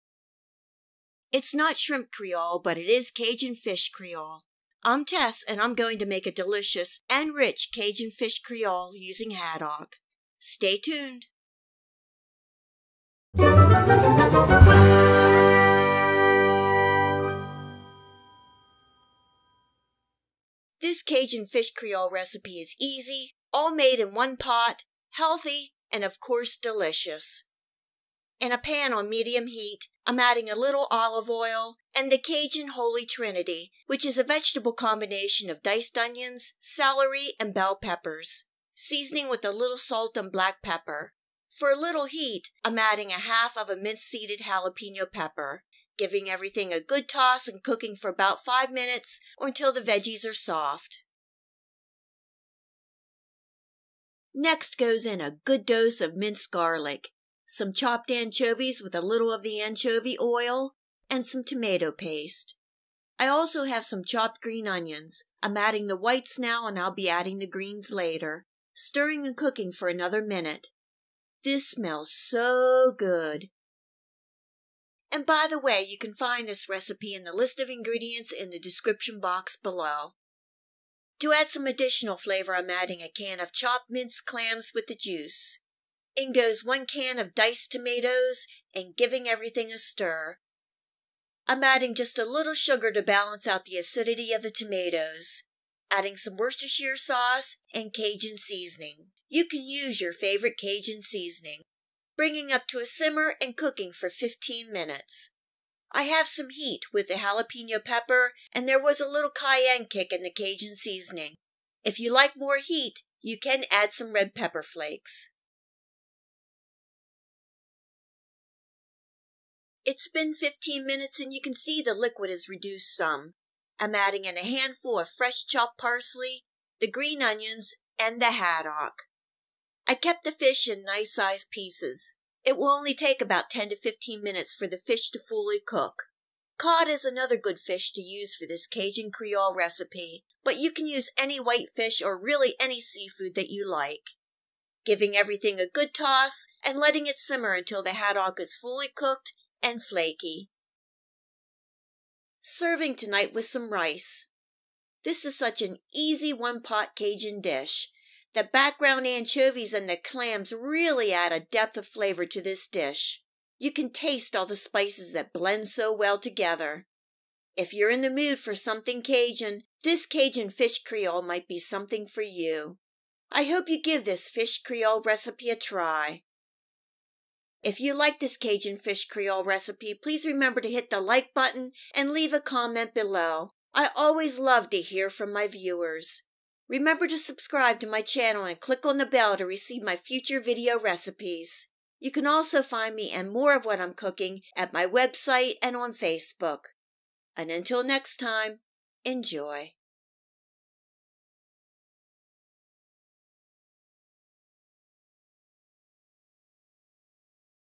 How to Make Easy Cajun Fish Creole ~ Cajun Seafood Creole Recipe
This is an easy and delicious Cajun fish creole recipe using haddock. This creole recipe is so flavorful and rich. You can use cod, any whitefish or seafood. I love all the Cajun creole spices. Next time, instead of making shrimp creole try making this easy Cajun fish creole recipe. Enjoy.

How to Make Easy Cajun Fish Creole

INGREDIENTS:
2 pounds Haddock
1 cup diced onions
1 cup diced celery
1 cup diced bell peppers
1/2 minced seeded jalapeno - optional OR you can
use red pepper flakes
1 tbsp. minced garlic
4 smashed anchovies with a little oil
1- 6.5oz. can chopped minced clams with juice
2 tsp. tomato paste
4 diced garden onions - whites and greens separated
1 28oz can diced tomatoes
2 tsp. sugar
1 tbsp. Worcestershire sauce
1 heaping tsp. Cajun spice
1/4 cup chopped fresh parsley
2-3 tbsp. olive oil

In a pan on medium heat add olive oil, onions, celery
bell peppers, jalapeno pepper (optional), salt and pepper.
Stir and cook for 4-5 minutes or until the veggies are soft.

Add minced garlic, anchovies/oil, tomato paste and whites
of garden onions. Stir and cook for an additional minute.

Add minced clams and juice and diced tomatoes.
Stir well.

Add sugar, Worcestershire sauce and Cajun seasonings.
Stir and bring up to a simmer.
Cook for 15 minutes. You can add some water or broth if
needed.

Add parsley, green onions and fish. Stir and cook
at a simmer for 10 to 15 minutes or until the fish is
cooked through and flaky.

Serve with rice or noodles.

Cuisinart Skillet

Pyrex 3-Piece Glass Measuring Cup Set

McCormick Cajun Seasoning

Lea & Perrins Worcestershire Sauce

Cento Tomato Paste in Tube